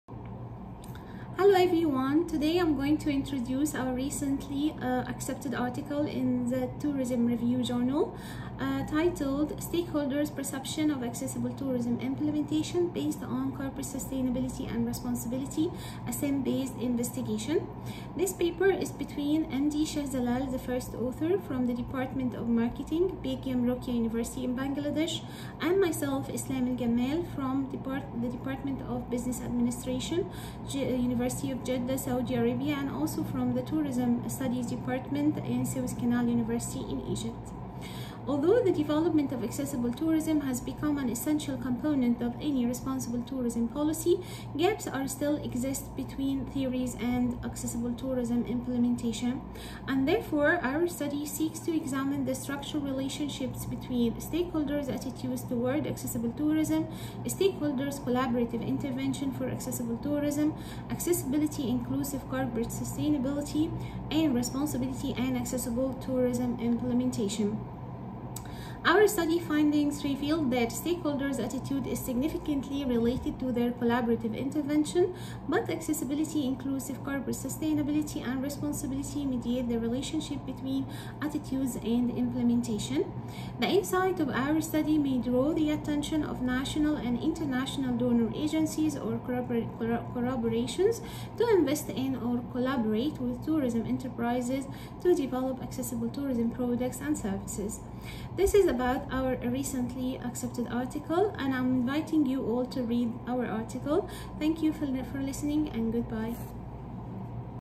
Stakeholders' perception of accessible tourism implementation
Purpose
In spite of the increased interest of academics, there has not been a thorough examination of the actors and factors of accessible tourism (AT) development. The purpose of this study is to investigate the relationships between stakeholders’ attitudes, collaborative intervention and corporate sustainability and responsibility (CS&R) and the implementation of accessible tourism (IAT).

Design/methodology/approach
Focus group discussion, an expert content validity test and structural equation modelling were all used as part of a mixed-methods approach. At the longest sea beach in the world, located in Bangladesh, data was collected from the tourism stakeholders using an on-site questionnaire survey.

Findings
Stakeholders’ attitude is significantly related to their collaborative intervention, but accessibility inclusive corporate substantiality and responsibility (AICSR) mediates the relationships between attitude and implementation, as well as collaborative intervention and implementation. As a result, AICSR is identified as a key driver in the IAT.

Research limitations/implications
Purposive sampling was used to select stakeholders from Bangladesh’s most popular tourist destination. Nonetheless, the statistically valid model of this exploratory study can be tested for generalization in other contexts with a large sample size. The findings will bring diverse stakeholders to light and encourage barrier-free tourist growth that could improve the destinations’ comparative advantage and competitiveness.

Originality/value
This study is, to the best of the authors’ knowledge, one of the first studies to use rigorous quantitative methods to investigate AT implementation with a focus on CS&R. It adds to the existing AT literature by providing a scale and model of IAT while highlighting the extent to which AT can be implemented within CS&R.